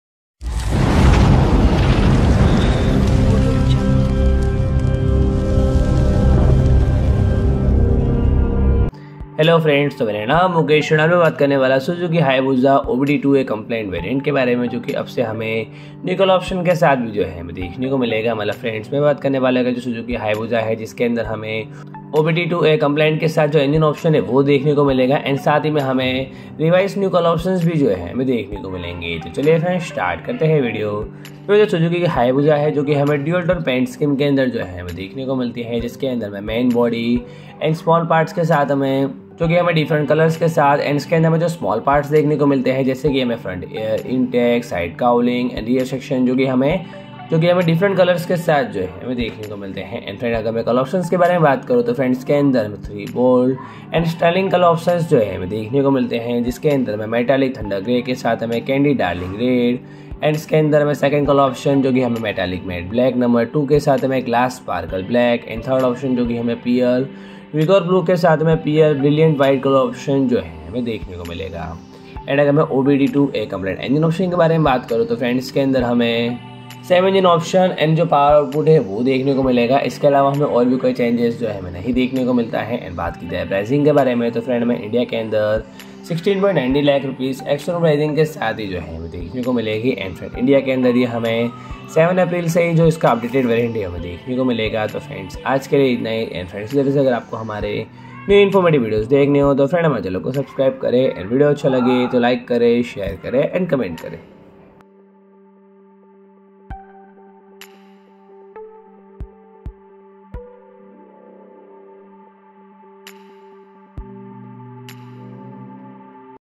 2023 Suzuki Hayabusa BS6 2.0 Variant Launched - Price From 16.90 Lakh - Explained All Spec, Features
Suzuki has launched the 2023 Hayabusa in India with a price tag of Rs 16.90 lakh, ex-showroom. This price is an increase of 49,000 over its previous pricing. Bookings for the motorcycle have commenced across the country. Now, the motorcycle gets the same update for the new model year as the international model. And these are the inclusion of new colours. You can have the Hayabusa in a striking Metallic Gray colour with Candy Red highlights on the front and side fairing along with the rear section. In similar accents, there is the Vigor Blue which comes with Pearl White as the main colour. Lastly, the 2023 Suzuki Hayabusa is also offered in a full-black paint option with grey lettering and a chrome strip on the side. But other than the new colour options, the Hayabusa is the same as the previous iteration. It continues to use a 1340cc, inline-four cylinder motor that churns out 190bhp and 142Nm. In addition to that, the electronics suite, as well as the braking and suspension hardware remains the same.